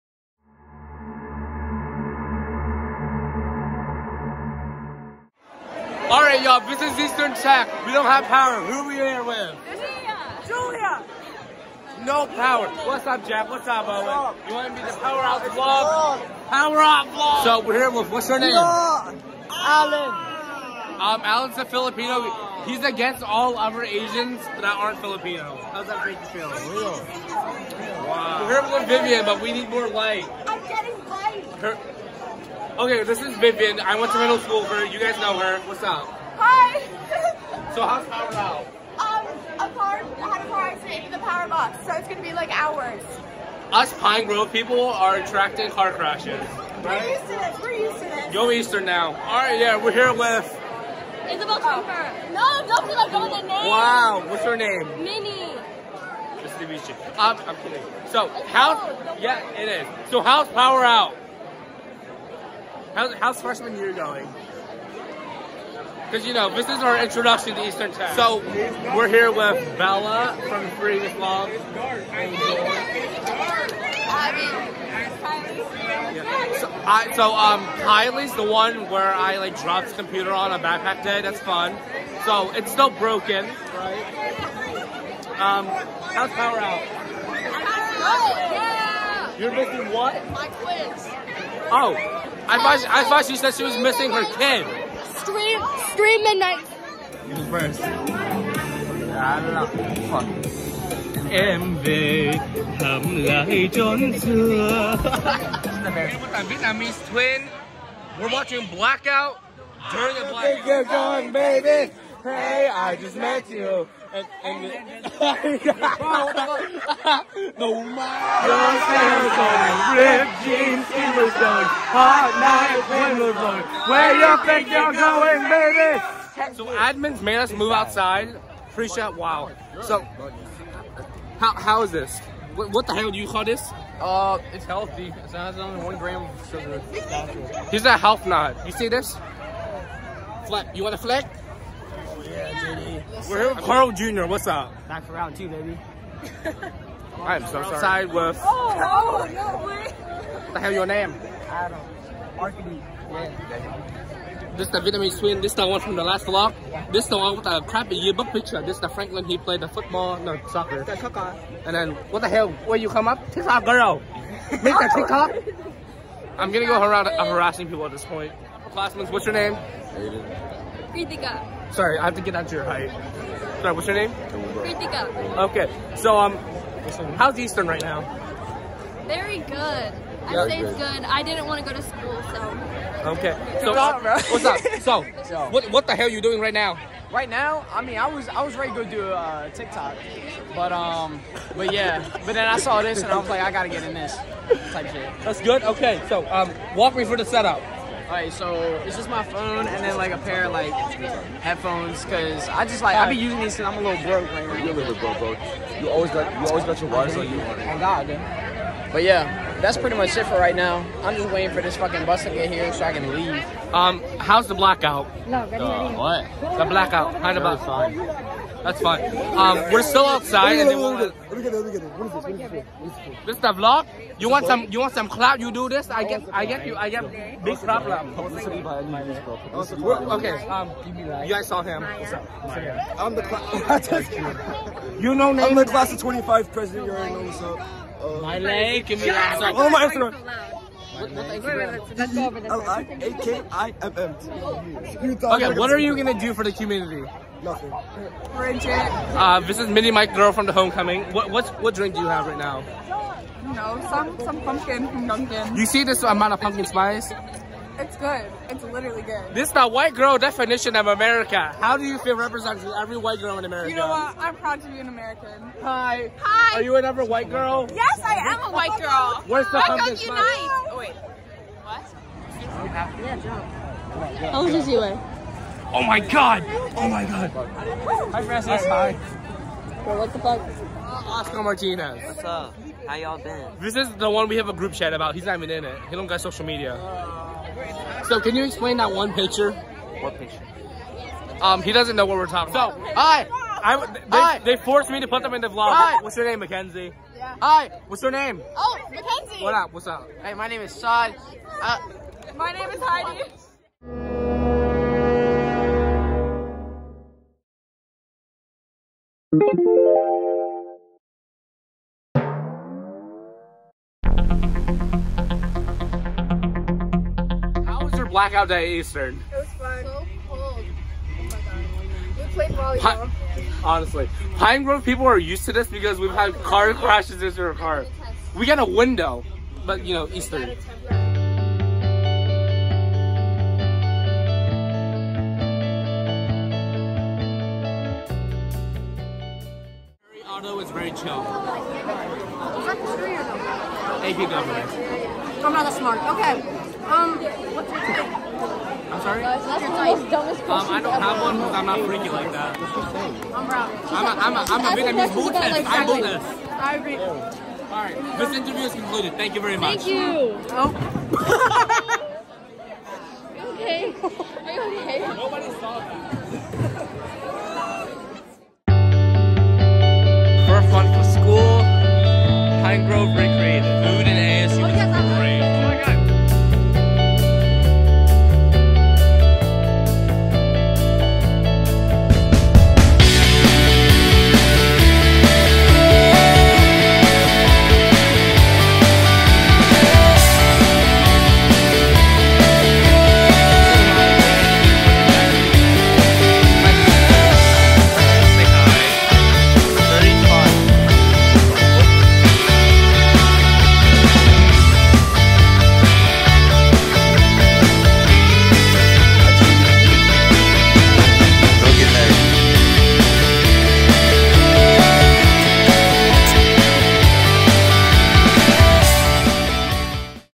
Eastern Tech 10-20 Blackout Vlog
On 10-20-22 We had a student motor vehicle accident that resulted in the loss of power at Eastern technical high school... Eventually the decision was made to dismiss students of the school day. This Vlog has a behind-the-scenes and a photo dump from this year so far. Enjoy!

All statements or jokes in this video or meant to be taken with a friendly attitude AND a grain of salt.

Be friendly in the comments :)